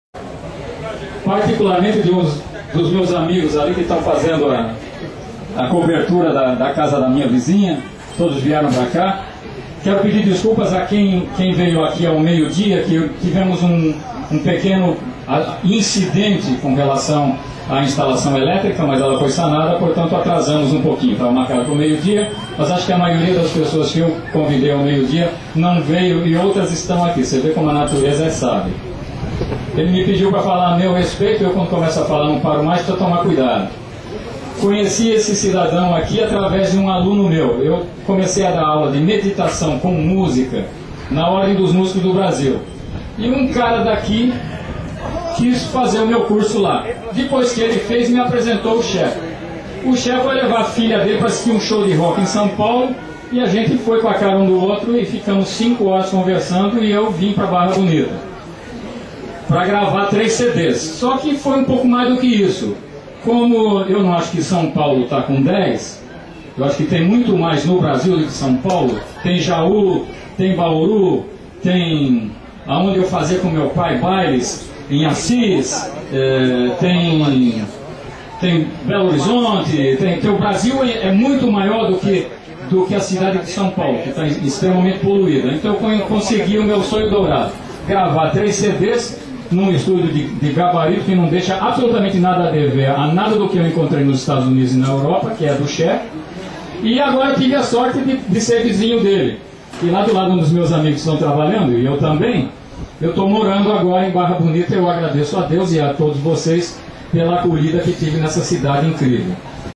anandtaza and ché costa interview in barra bonita's festival
COMO EU VIM PARAR EM BARRA BONITA, EXPLICAÇÃO e amizade com Ché Coista, organizador desse tremendo brilhante evento magnífico, de 20 horas ininterruptas de música ao largo do Rio Tiete, na Praça do Museu de Barra Bonita monitorado pela linda Janaina e seu namorado Vitor.
Filmado por uma amante das artes improvisado, com a minha magnífica câmera SONY.